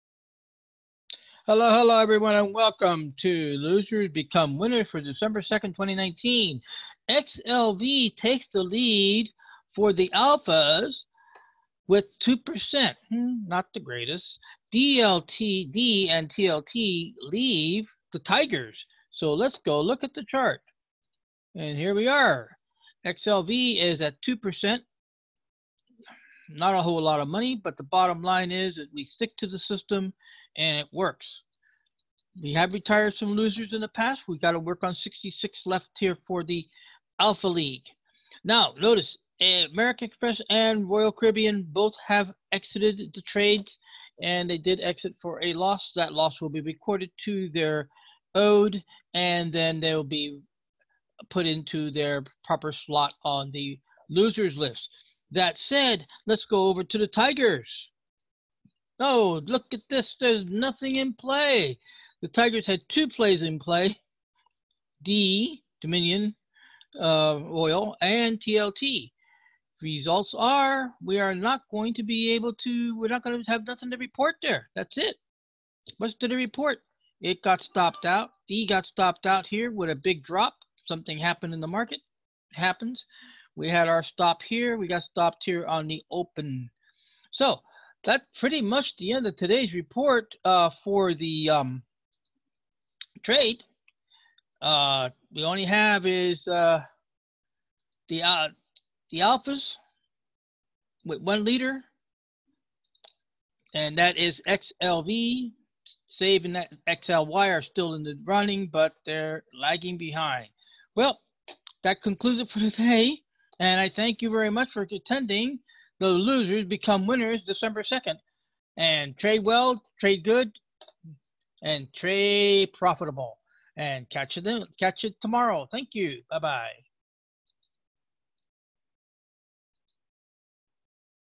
Losers Become Winners Dec 2 2019 XLV takes the lead
XLV takes the lead for the Alphas as D & TLT exit the Tigers.